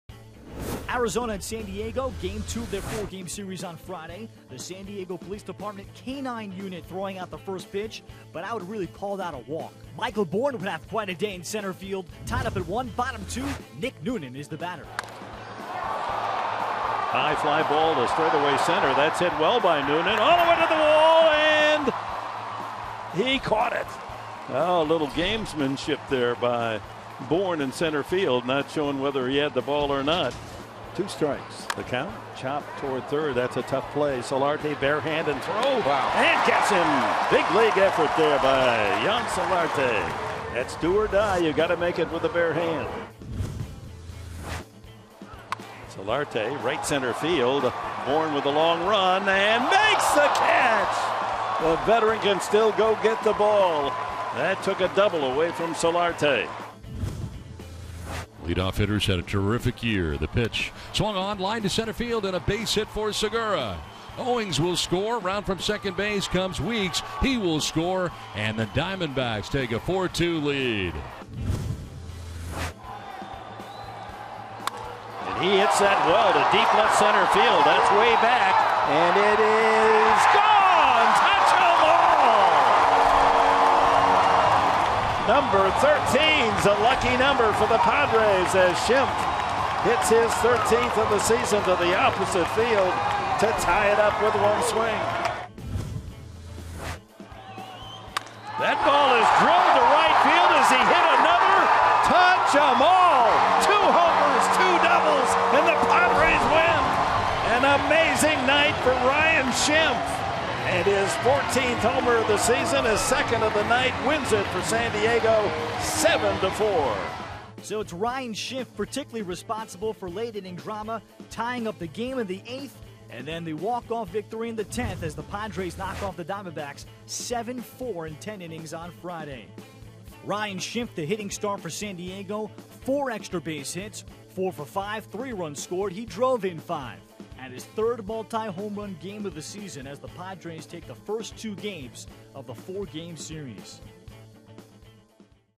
8/19/16: Schimpf's homers lift Padres to walk-off win
Daily Recap: After Ryan Schimpf smashed a game-tying two-run homer in the 8th, he won the game with a walk-off three-run homer in the 10th